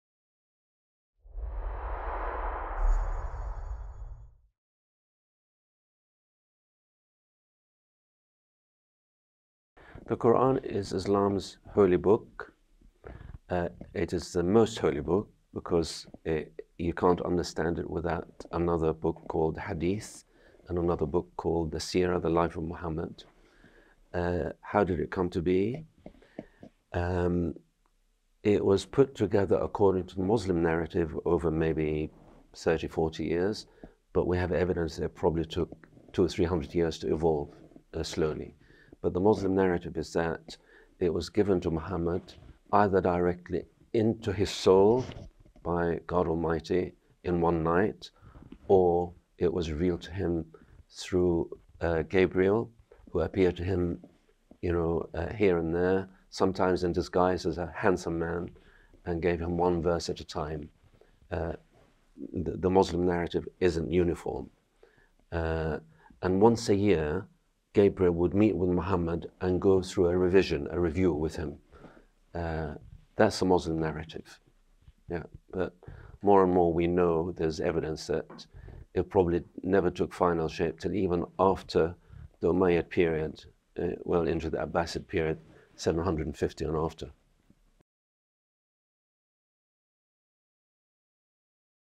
What is the Qur'an and how did it come to be? - Emil Shehadeh
Emil Shehadeh was born in Israel and educated mainly in the UK, achieving BSc Hons (Medical Biology), MSc (Biochemistry), MB ChB (Medicine), and a Diploma in Dermatology and MRCGP. Upon retirement, he dedicated his time to writing and speaking on the subject of Islam and evangelising Muslims. Emil has a PhD in Islamic Studies from Logos University and is engaged in another piece of research at LST, Northwood, on the Causes and Consequences of Muslim Conversions to Christianity. Here, Emil answers questions from the talk "Scrutinising Claims Muslims Make About the Qur'an", given at the 2023 European Leadership Forum.